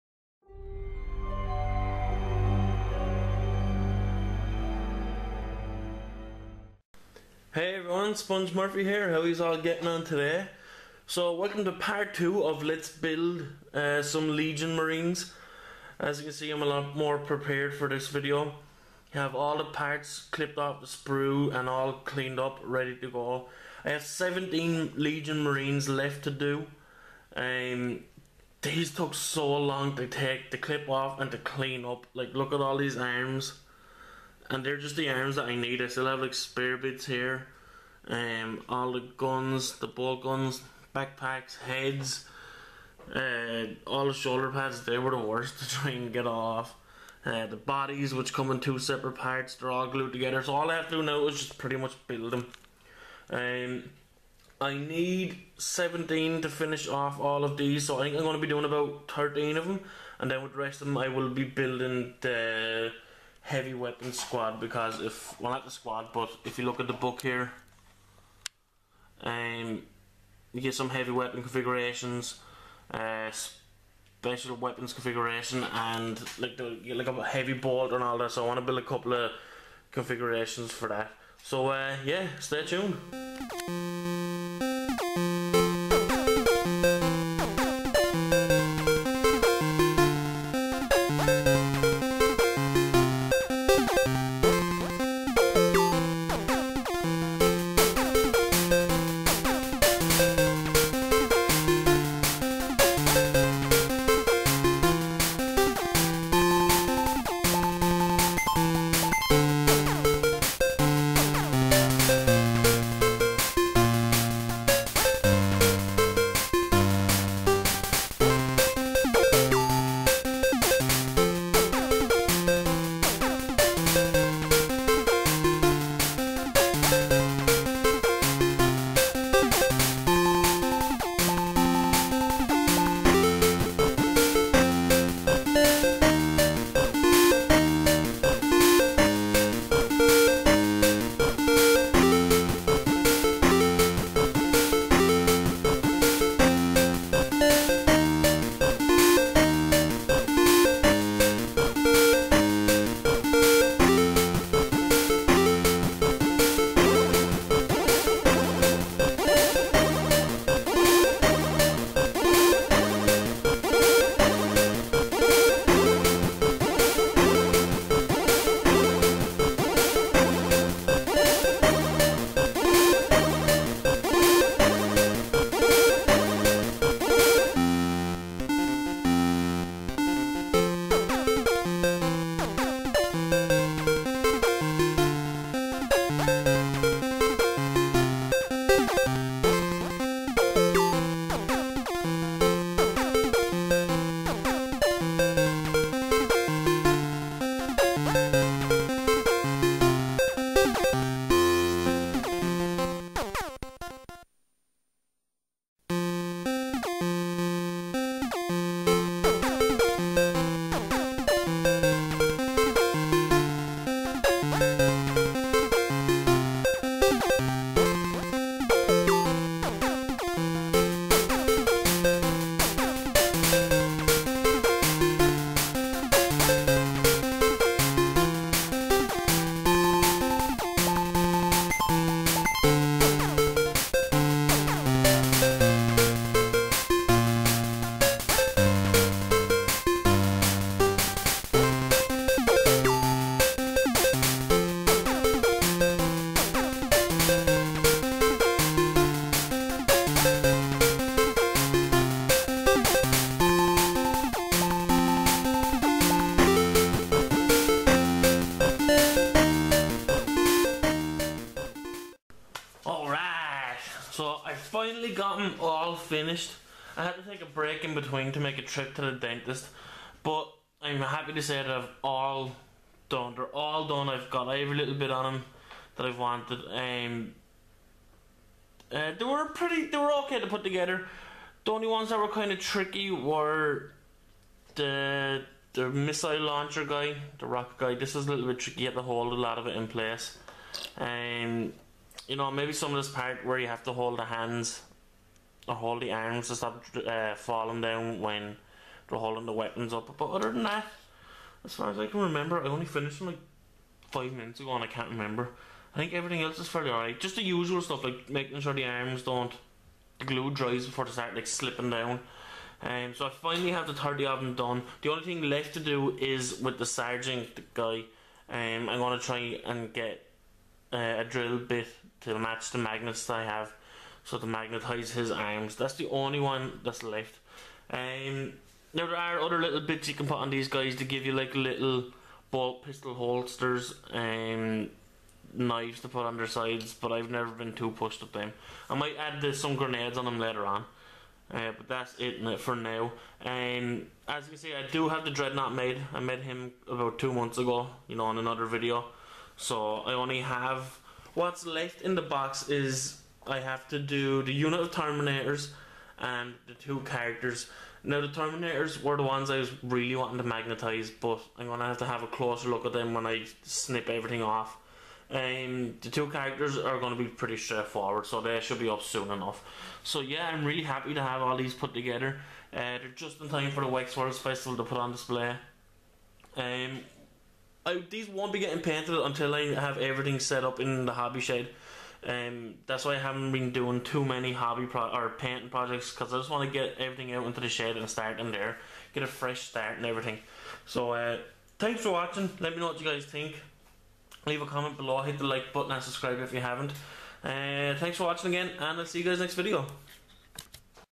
Lets build 30 Legion Space Marines Part 2
And we are finished! 3o new space marines for the collection!